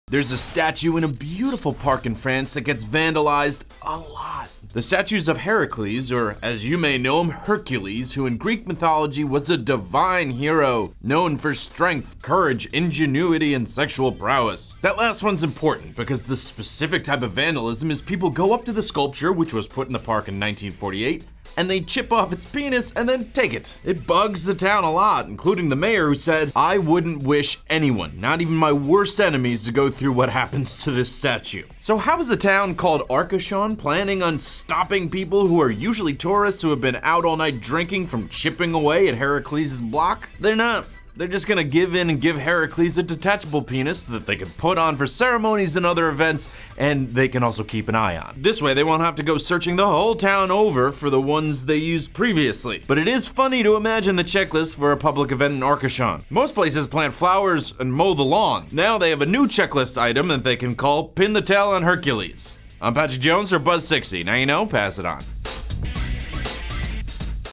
Town to Give Statue Detachable Penis to Stop People Stealing It
A statue of Heracles in the French town of Arcachon is vandalized reasonably often and the people there are fed up. They've come up with a solution though. Patrick Jones (@Patrick_E_Jones) explains.